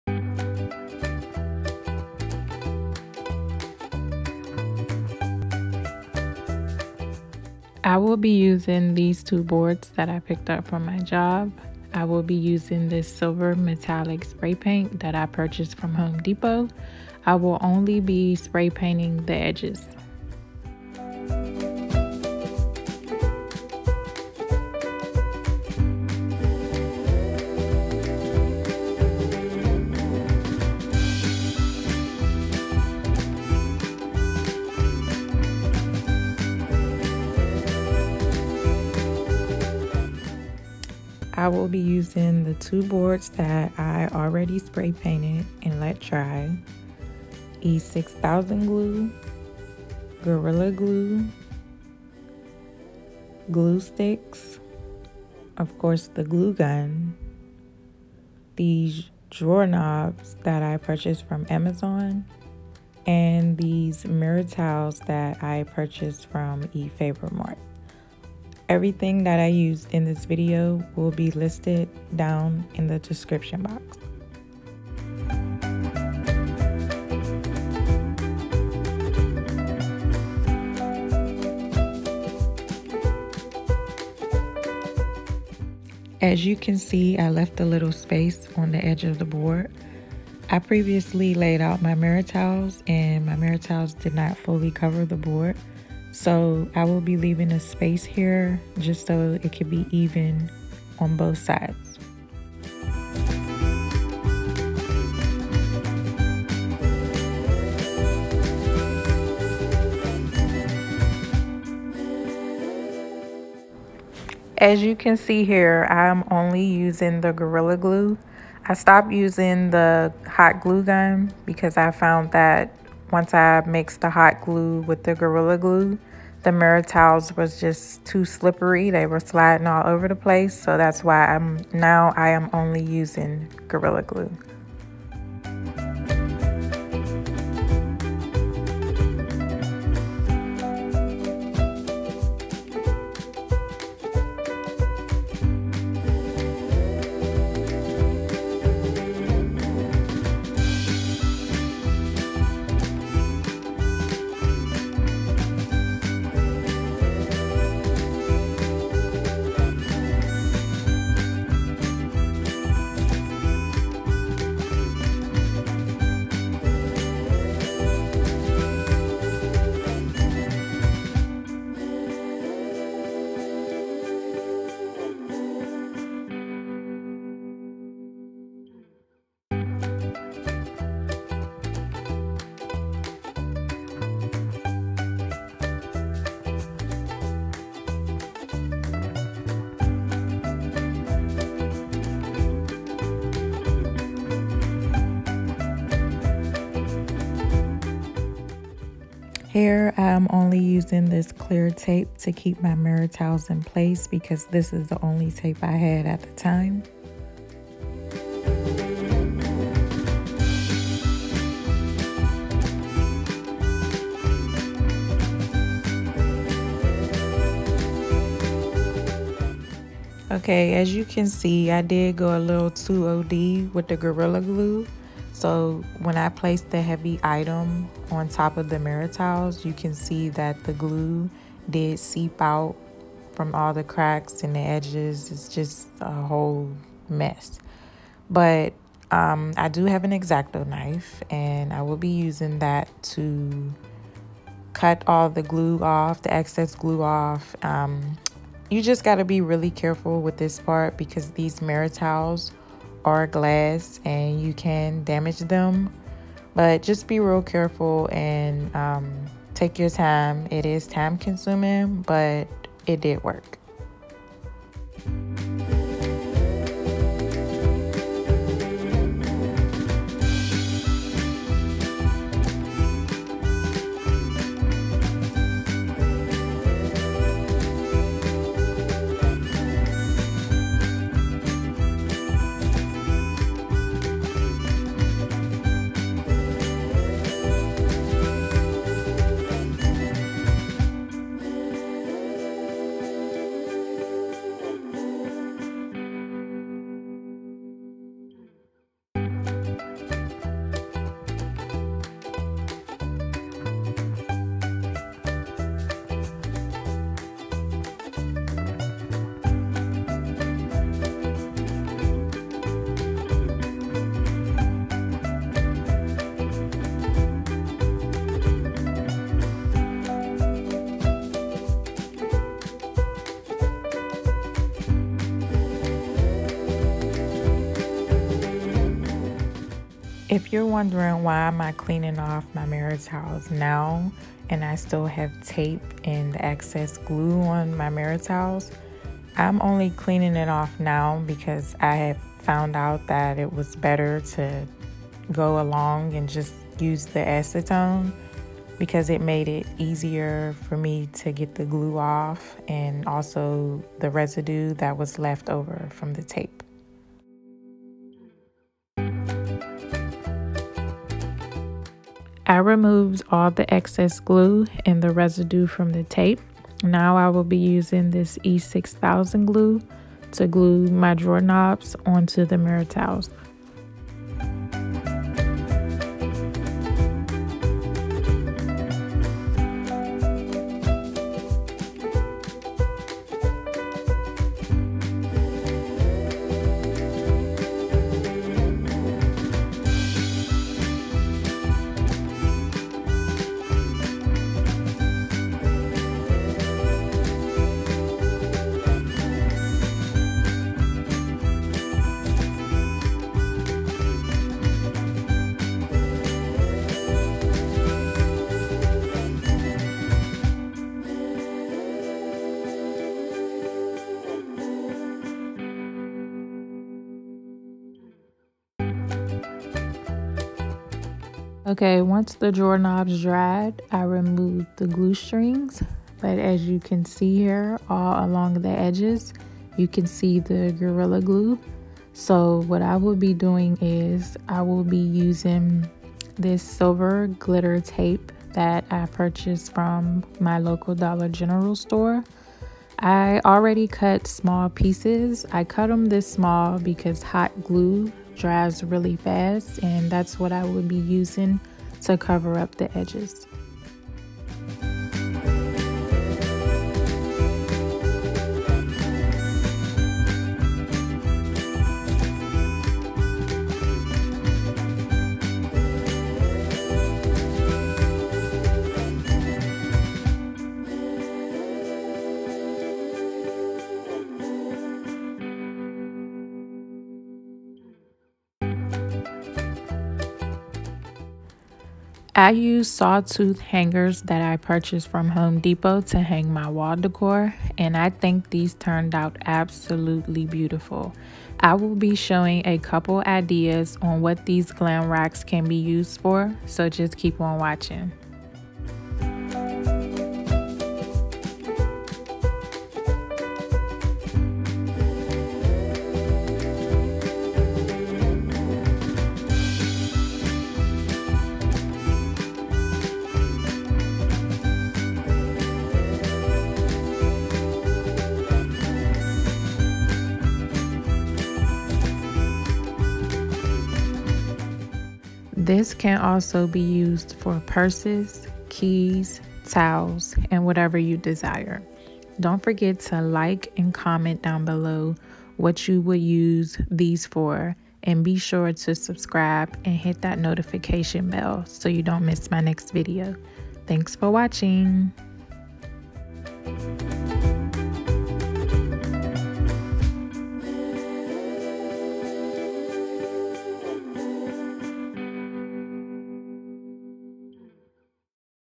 Easy Coat Rack DIY Idea For Cheap | Glam Purse Rack🤍
I will be showing you how I made this Easy Coat Rack DIY Idea For Cheap. It can also be used as a Glam Purse Rack/Coat Rack (etc). I  used free wood and Mirror Tiles from efavormart. This diy project was super easy and affordable to make. It brings a unique look to my home!

✨Materiales Used:

Reused Wood
Drawer Knobs from (Amazon)
Mirror Tiles from (efavormart)
E-6000 Glue (Dollar General)
Glue Sticks/Glue Gun (Dollar General)
Metallic Silver Spray Paint (Home Depot)
Gorilla Wood Glue (Dollar General)
Silver Glitter Tape (Dollar General)
Exacto Knife (Michaels)
Cotton Balls/Acetone (Dollar Tree)